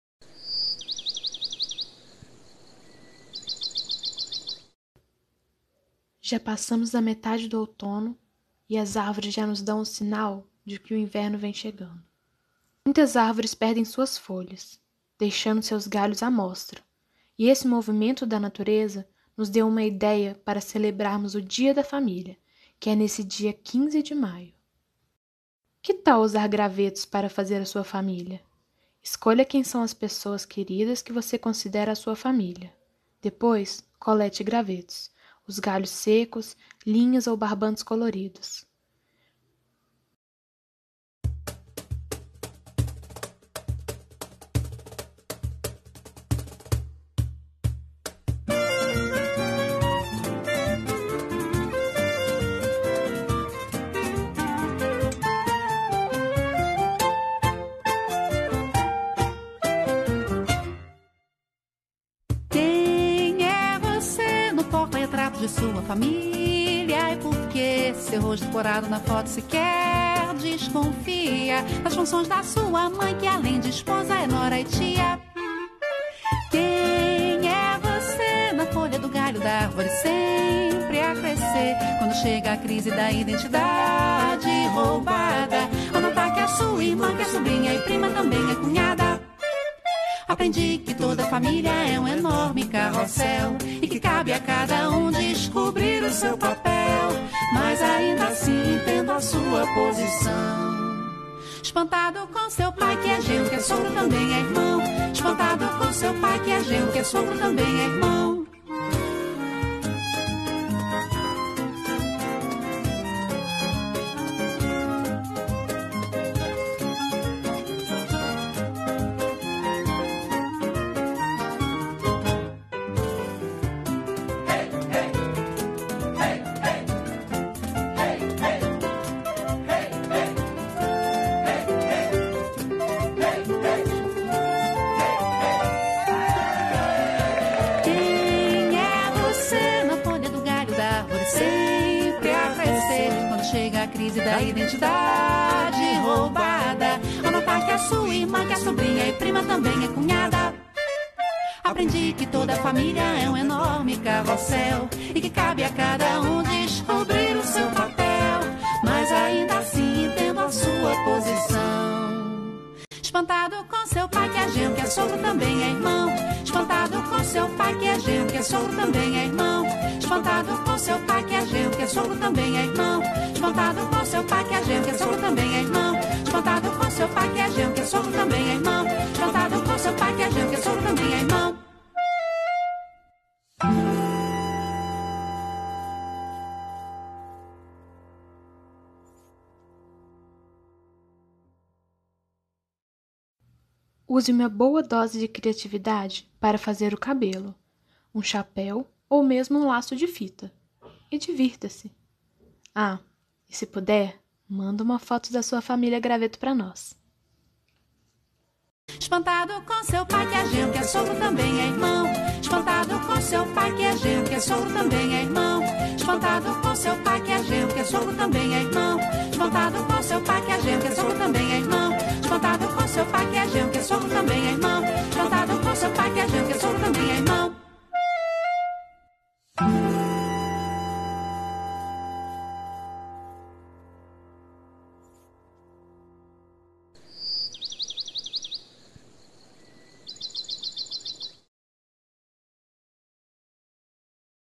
Familia graveto!
Que tal passear em família junto à natureza e procurar galhos secos para nossa atividade desta Sexta Casulo? Pelo vídeo, que vem acompanhada de uma canção bem divertida do Grupo Cria, você já pode começar. E viva a sua família, do jeitinho que ela é! 👩‍👧‍👦👨‍👧👨‍👨‍👦‍👦 👩‍👩‍👧‍👧 👨‍👩‍👧